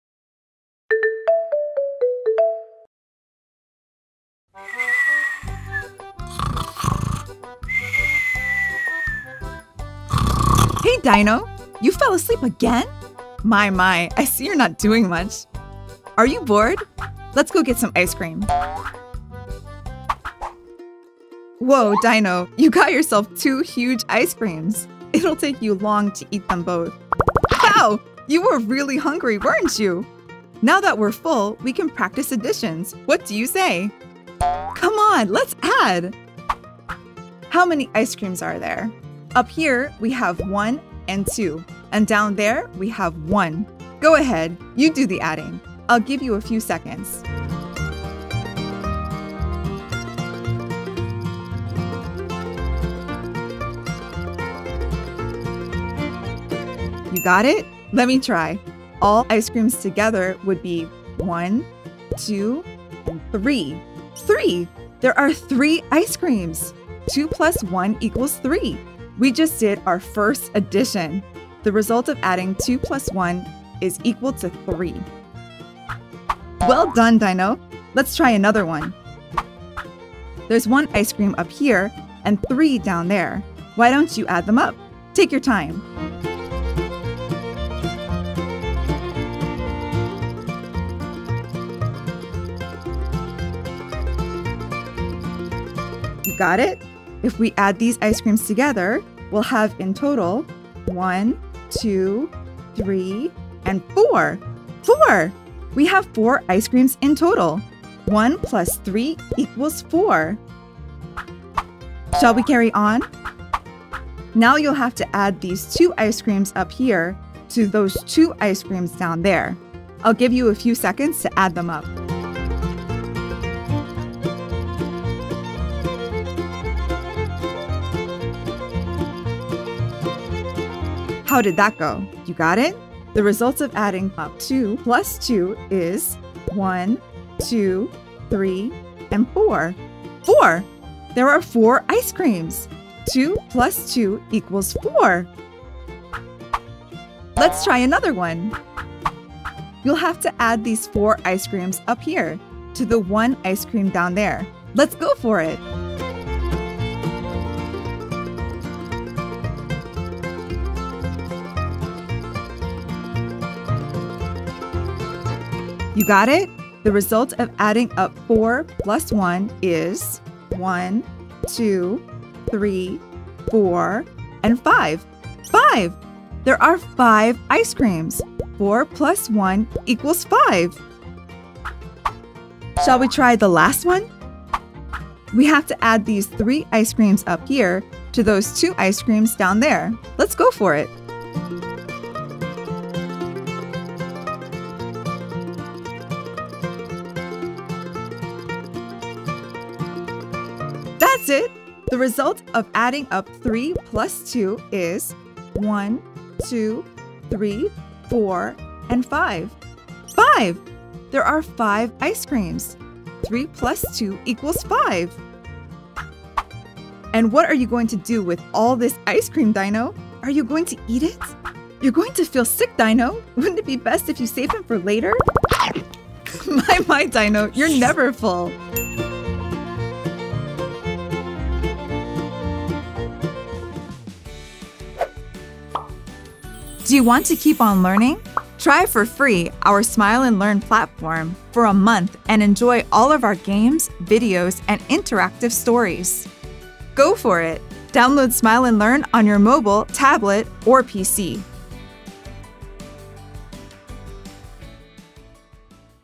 Addition exercises for kids - Learn to add with Dino - Mathematics for kids
Educational video for kids to practise Mathematics and to learn how to add with Dino, the dinosaur. Take your time to do the math operation at home or in school and check your results within seconds!

This video is part of a Math videos collection for kids to learn how to do mathematical operations and to sparkle their interest in this school subject. Recommended for Preschool Education.